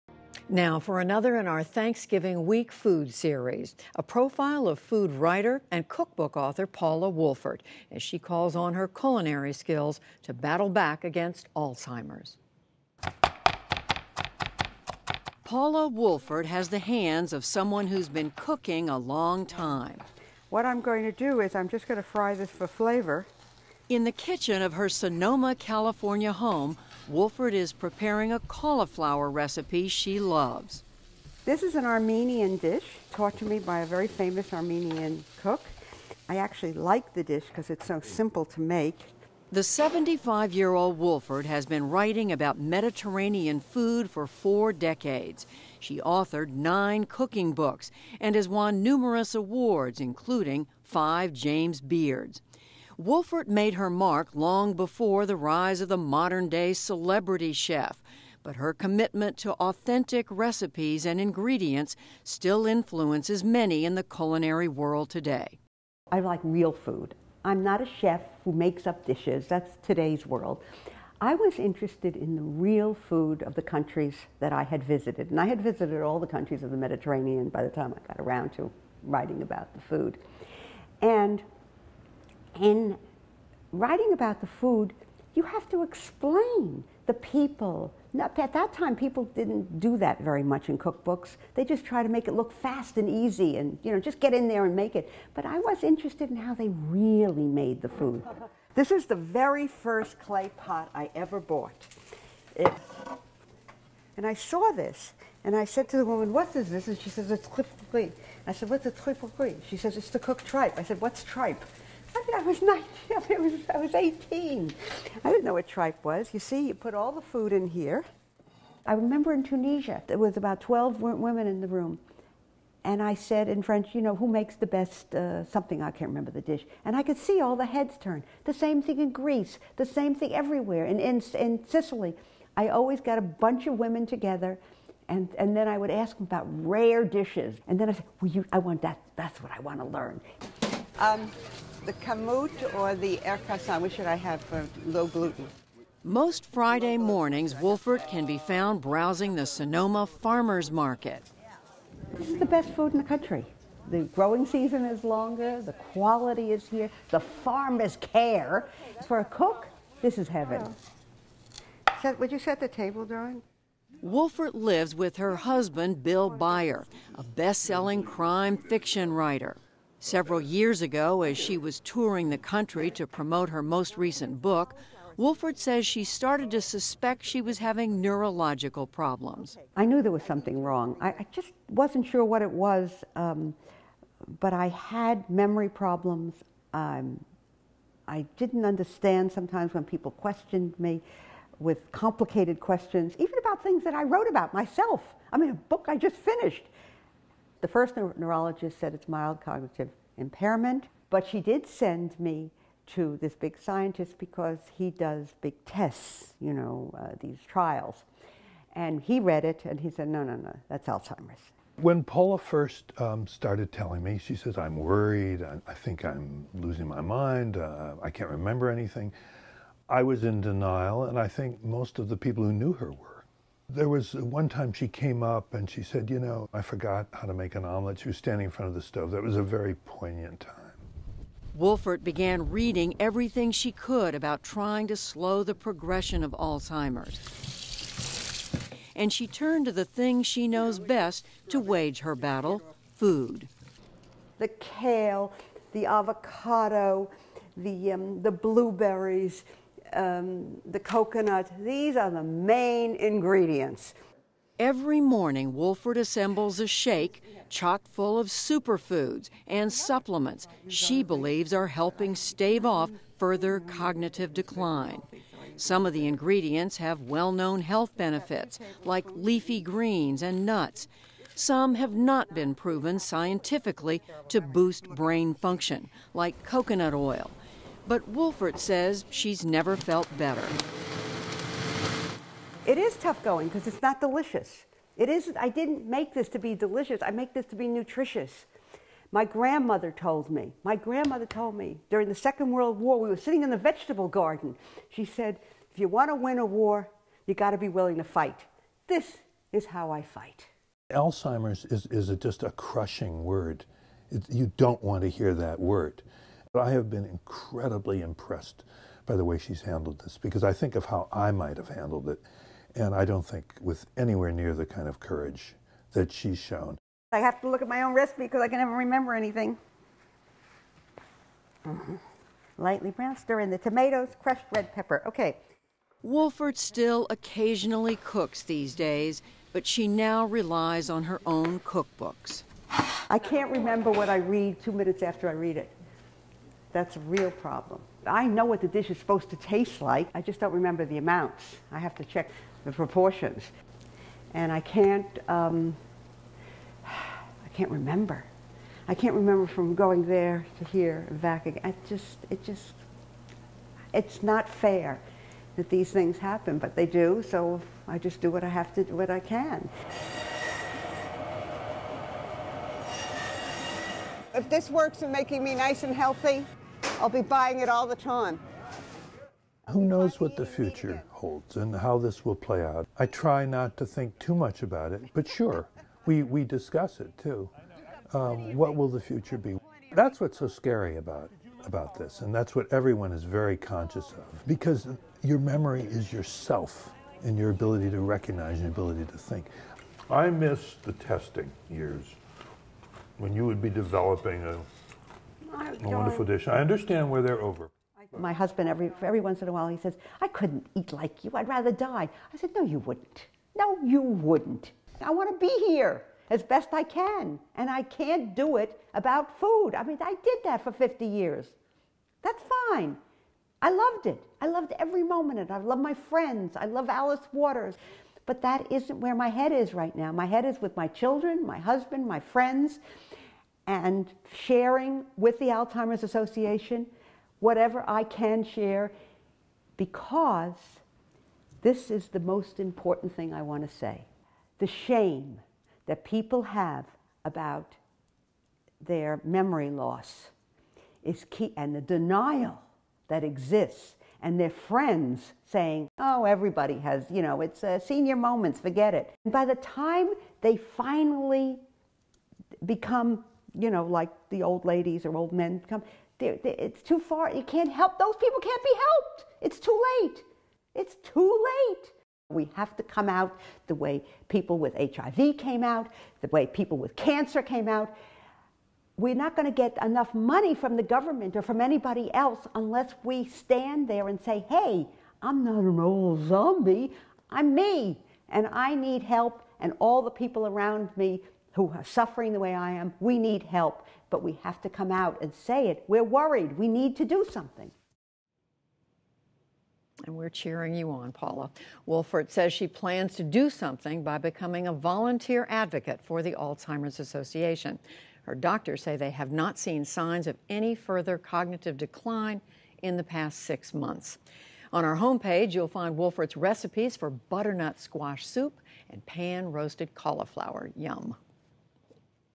Food writer Paula Wolfert cooking to cope with Alzheimer's
Award-winning cookbook author Paula Wolfert can't remember all of the ingredients in her recipes anymore, but she still knows how her dishes should taste. Hoping to delay the effects of Alzheimer's disease, Wolfert copes by continuing to cook. Judy Woodruff reports on why Wolfert wants more people to declare their memory loss.